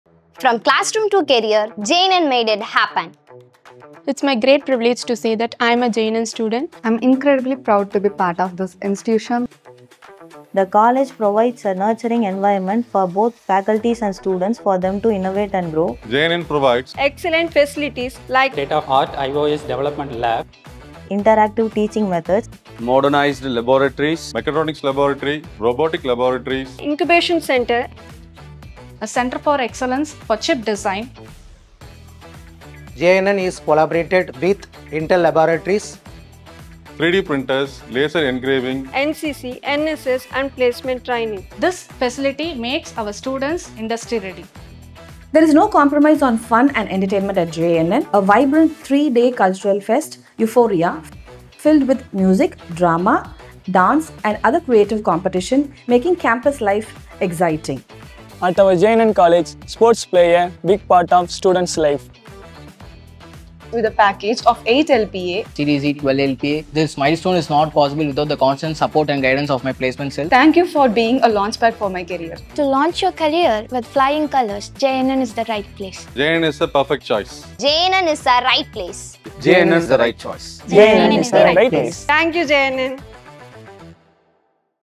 Students & Faculty Say THIS About J.N.N 🎓✨
What makes J.N.N the right choice for your future in Engineering or Arts & Science? 🎓 @jnncollege
Hear it straight from the hearts of our students and faculty of J.N.N Institute of Engineering and J.N.N College of Arts & Science — where passion meets purpose. 💡

✅ Real stories
✅ Real people
✅ Real impact

Join the journey. Join J.N.N.